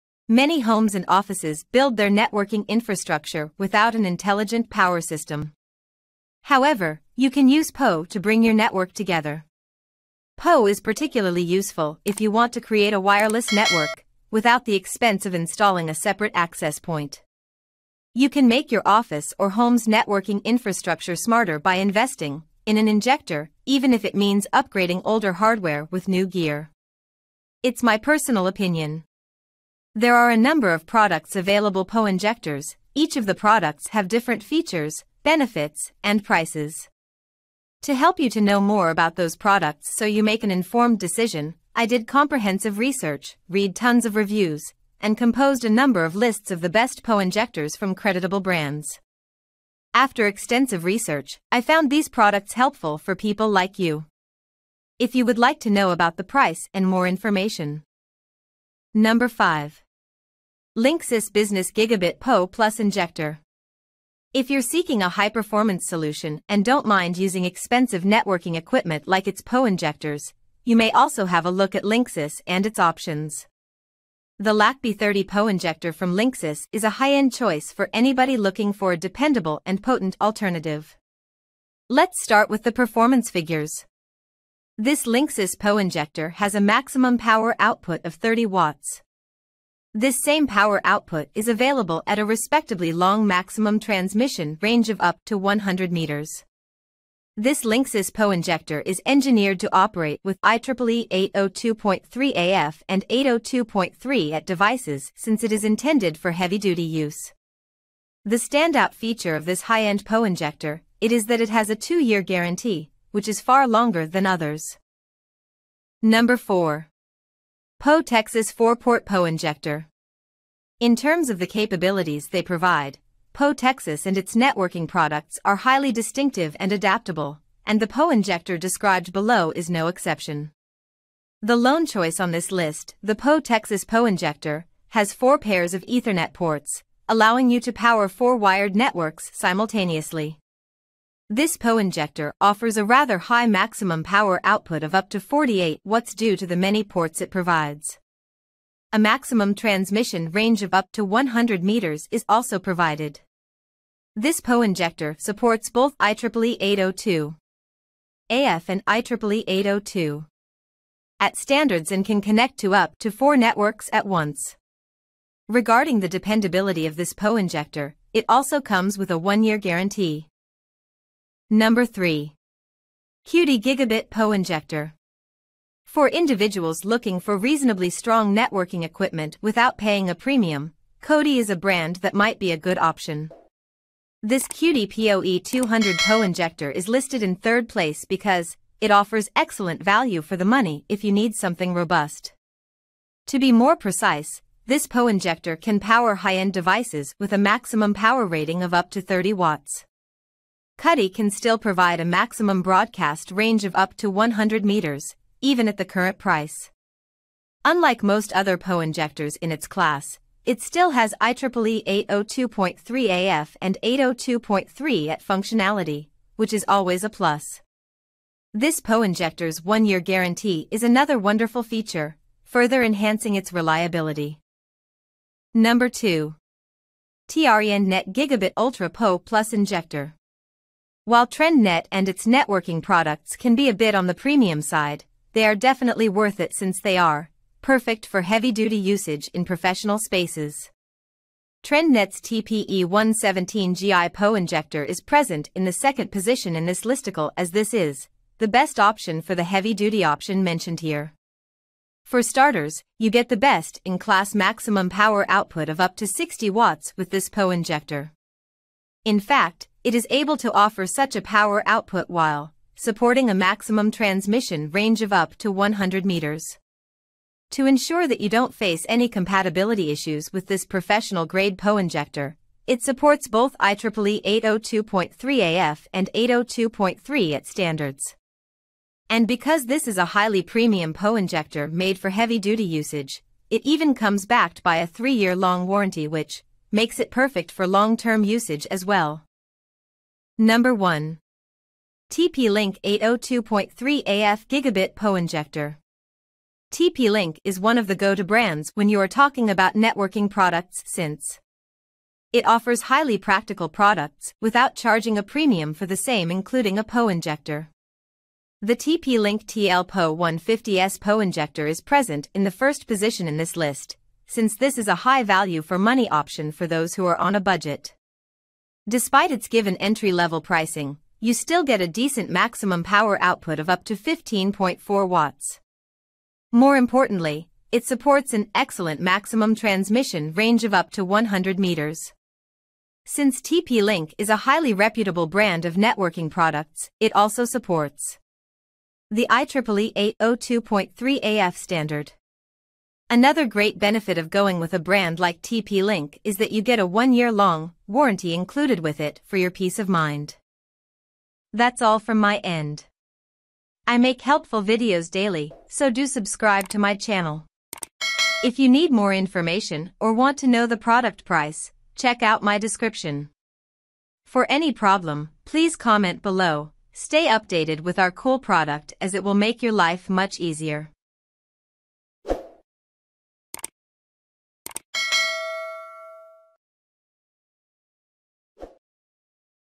5 Best Poe Injectors Reviews | Linksys Business Gigabit High Power Poe Injector
5 Best Poe Injectors Reviews | Linksys Business Gigabit High Power Poe Injector

Hey guys,
in this video we are going to be checking out the best product.

👇👇Product Link👇👇

1. TP Link Gigabit Poe Injector
2. TRENDnet Gigabit Ultra Poe Injector
3. Cody POE200 30W Gigabit Poe Injector Adapter
4. Poe Texas 4 Port Poe Injector
5. Linksys Business Gigabit High Power Poe Injector

Looking for a reliable and powerful PoE injector to power your network devices? Look no further than the Linksys Business Gigabit High Power PoE Injector. In this video, we will review the top 5 best PoE injectors on the market, including the Linksys Business Gigabit High Power PoE Injector.

First on our list is the Ubiquiti Networks PoE Injector. With a compact design and easy setup, this injector is perfect for powering access points, IP cameras, and other network devices. It provides reliable and efficient power delivery, and comes with surge protection for added safety.

Next up is the TP-Link PoE Injector. This injector is designed with 802.3af PoE standards, providing up to 15.4W of power per port. It's compatible with a wide range of devices, including IP phones, wireless access points, and IP cameras.

Third on our list is the Netgear PoE Injector. This injector delivers up to 30W of power per port, making it ideal for powering high-powered devices like pan-tilt-zoom cameras and access points. It's easy to install and features LED indicators for monitoring power status.

Fourth on our list is the Trendnet PoE Injector. This injector supports both 802.3af and 802.3at PoE standards, delivering up to 30W of power per port. It's perfect for powering PoE devices like access points, IP cameras, and VoIP phones.

Finally, we have the Linksys Business Gigabit High Power PoE Injector. This injector provides up to 30W of power per port, making it ideal for powering high-powered devices like access points and IP cameras. It features surge protection, and supports both 802.3af and 802.3at PoE standards.

In conclusion, if you're in need of a reliable and powerful PoE injector, any of these five options are great choices. However, if you're looking for the best of the best, the Linksys Business Gigabit High Power PoE Injector is the way to go.